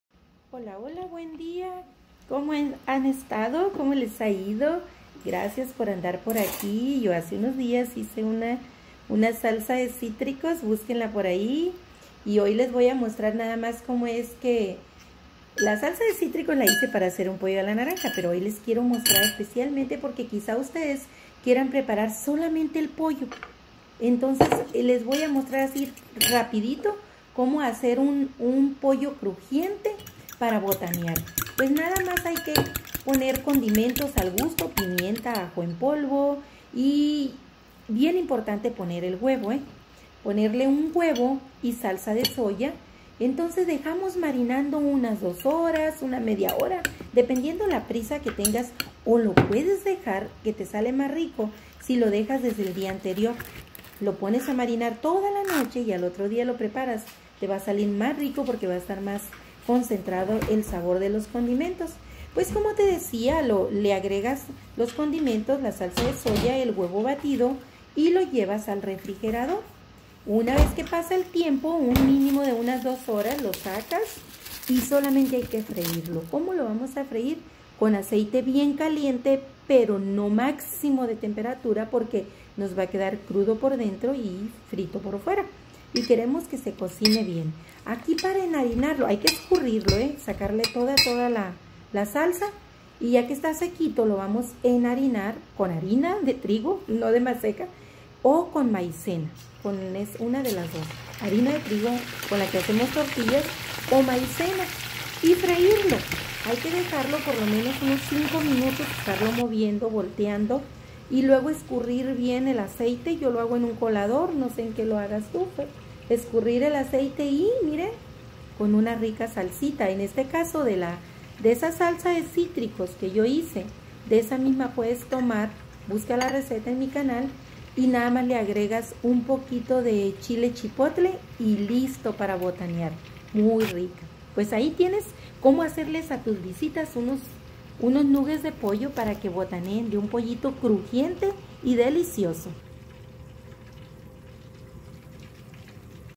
Pollo Crujiente Para Botanear! - SinPretextos.
Para preparar unos nuggets pollo para botanear se necesitan los siguientes ingredientes:
1 Pechuga de pollo en trocitos cortados del mismo tamaño.
1 cucharada de salsa de soja.
1/2 cucharadita de ajo en polvo.
1 pizca de pimienta.
1 huevo completo.
Marinar el pollo mínimo 2 horas preferible toda la noche en el refri.
Posteriormente escurrir la salsa y enharinarlo.
Freírlo en aceite caliente cuidando que no quede crudo por dentro.
Servirlo con una rica salsa o aderezo de cilantro y limón.
NOTA con este pollo puedes hacer el pollo enchilado y el pollo a la naranja.
GRUPO DE FACEBOOK    "COCINA RICO SIN PRETEXTOS"
PÁGINA DE FACEBOOK  "COCINA RICO CON HUERTO EN CASA"